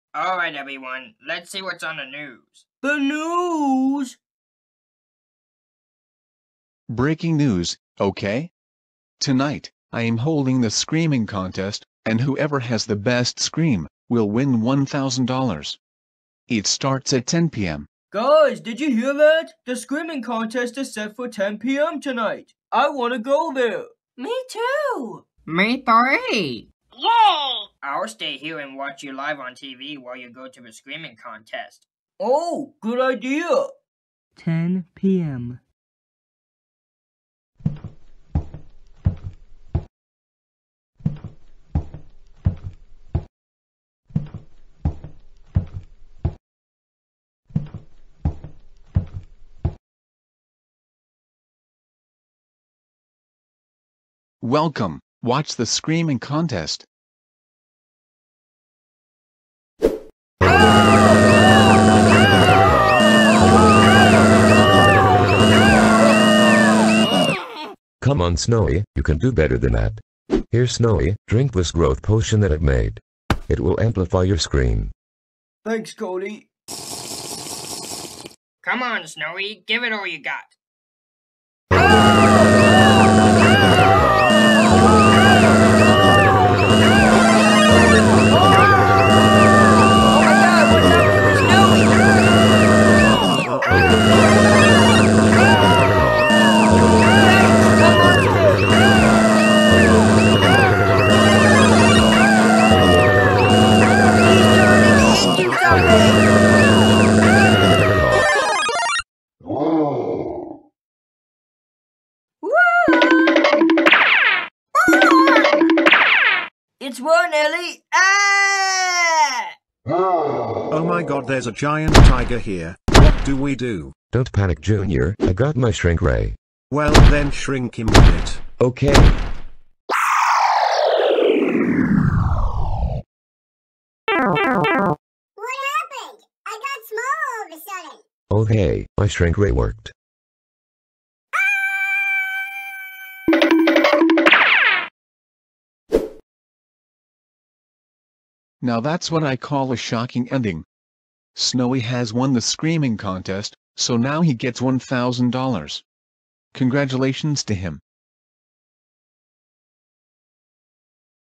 Choco and Snowy: The Screaming Contest
The animals go to the screaming contest.